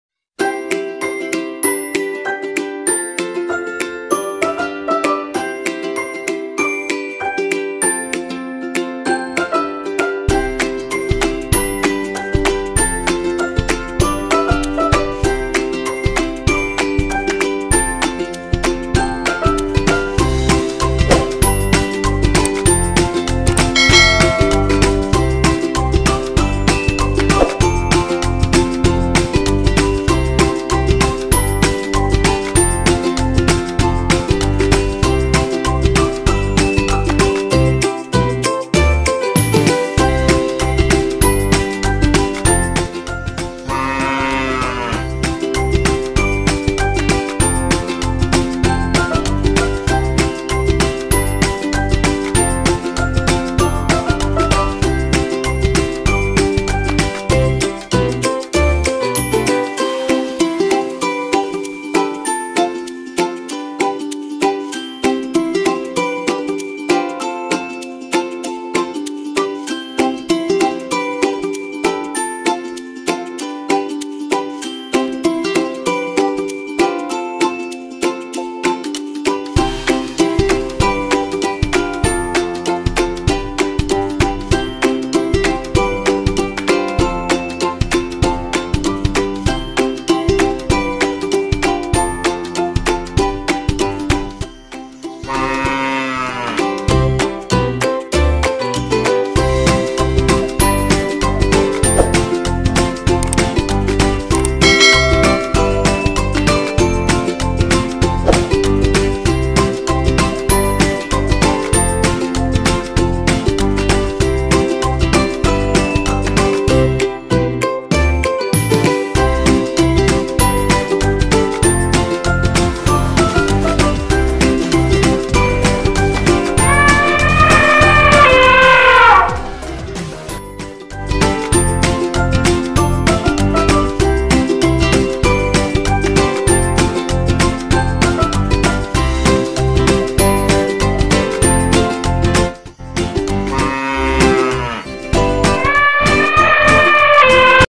Puzzle Sapi Dan Gajah || Menyusun Puzzle Sapi dan Gajah
Menyusun Puzzle Hewan 🐄🐮 Sapi dan 🐘🐘 Gajah

Hallo Kawan kawan...

Di video kali ini kita akan menyusun sebuah Puzzle Hewan Sapi dan Gajah

Bermain Puzzle juga dapat membantu :
-Merangsang otak anak
-Berfikir memecahkan masalah
-Mengembangkan koordinasi mata dan tangan anak
-Keterampilan motorik dan kognitif
-Melatih kesabaran anak
- Melatih daya ingat anak

Terima Kasih